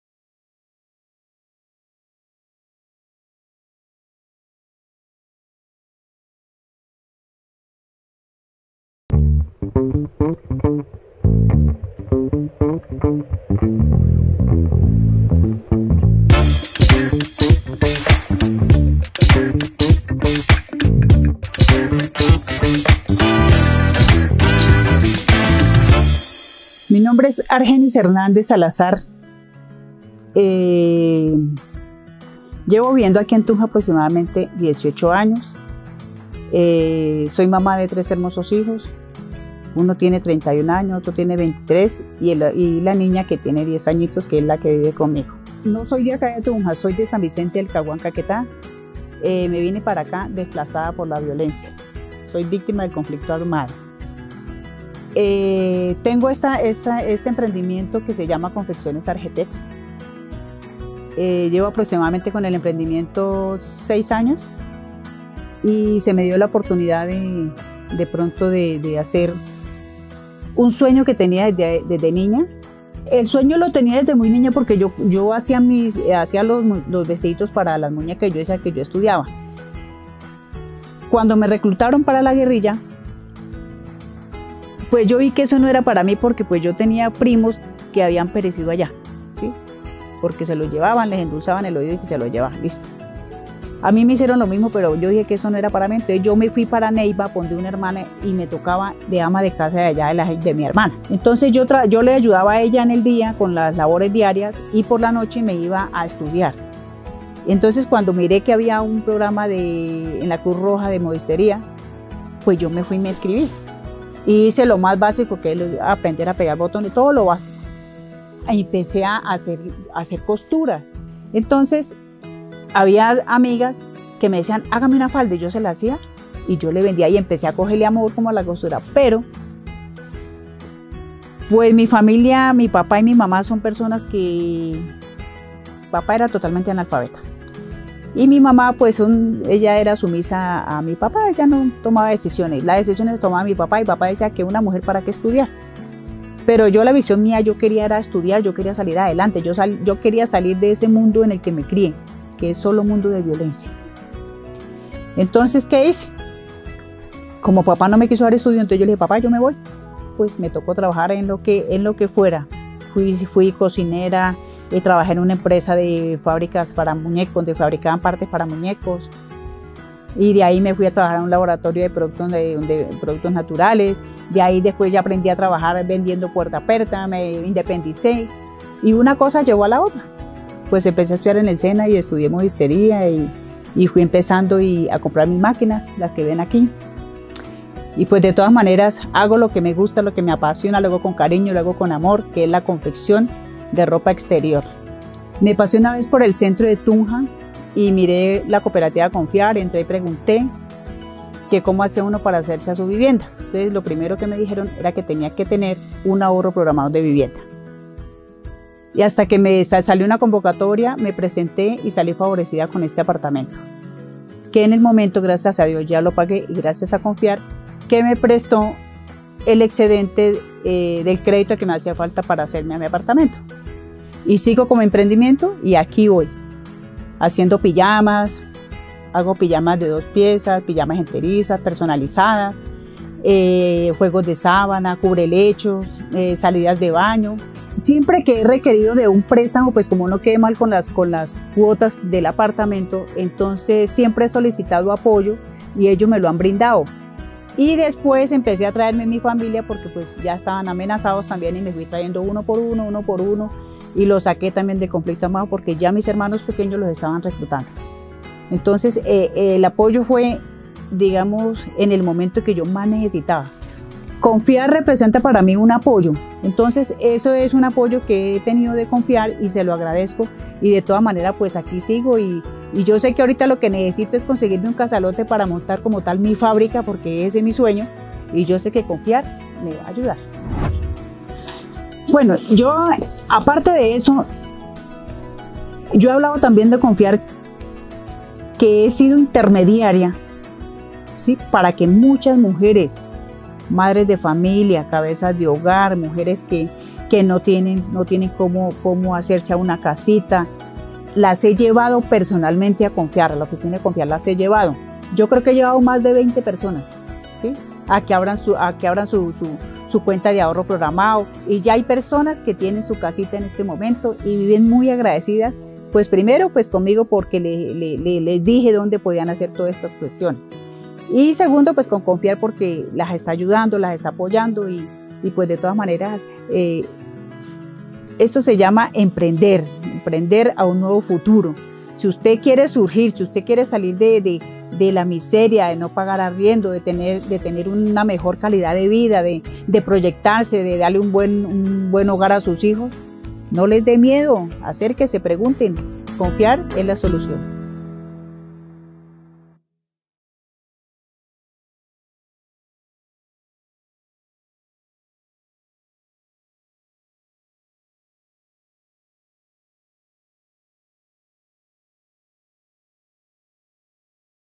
Argenis Hernández | Asociada de Tunja Confiar Cooperativa Financiera
En OtroMundoPosible les contamos la historia de Argenis Hernández, asociada de Confiar en Tunja desde 2012. La cooperativa la ha acompañado en su propósito de ampliar su negocio de confecciones y venta de pijamas. Conoce su historia en este video